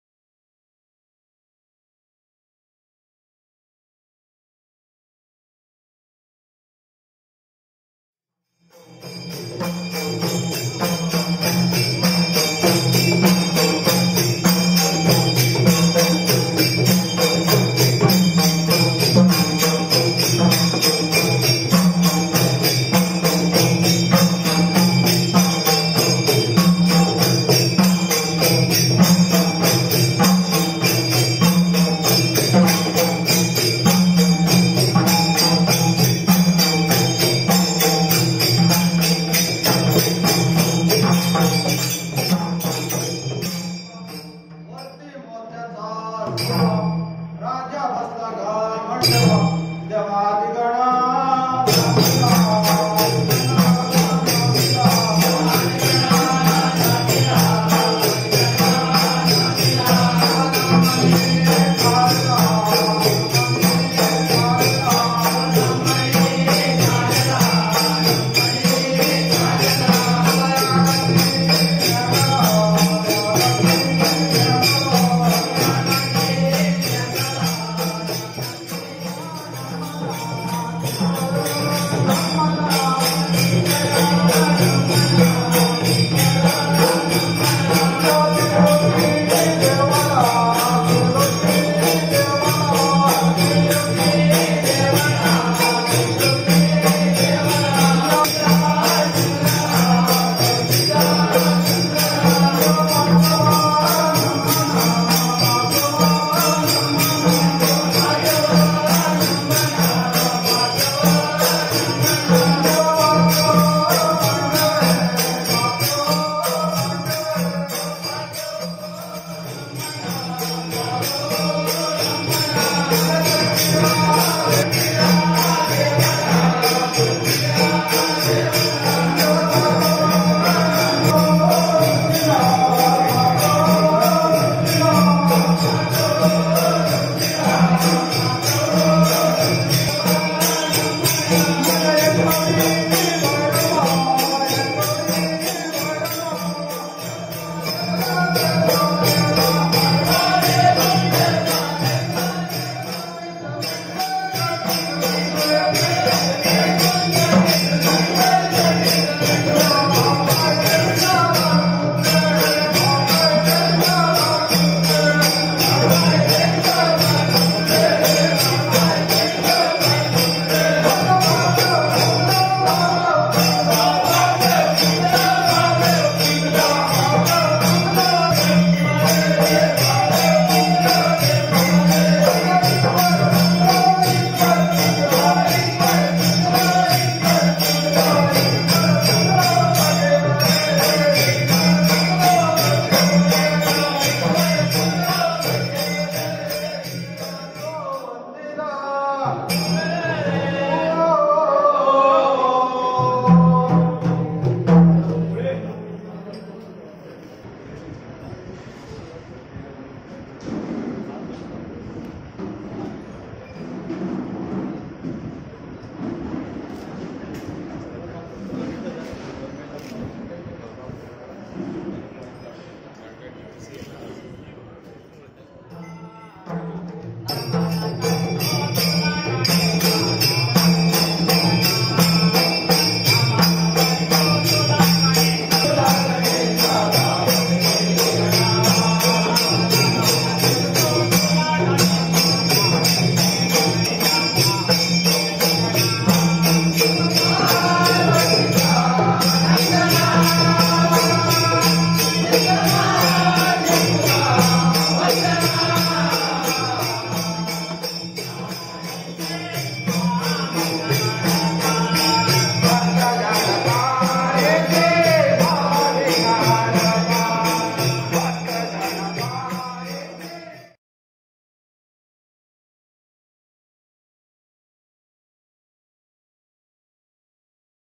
रत्नागिरी जिल्ह्यातील पारंपरिक खेळे - नमन
रत्नागिरी पर्यटन सहकारी सेवा संस्थेने आयोजित केलेली वारसा पर्यटन या विषयावरील तिसरी शाश्वत पर्यटन परिषद २७ जानेवारी रोजी रत्नागिरीत झाली. पारंपरिक खेळ्यांनी परिषदेचा प्रारंभ झाला.

कोकणातील लोककला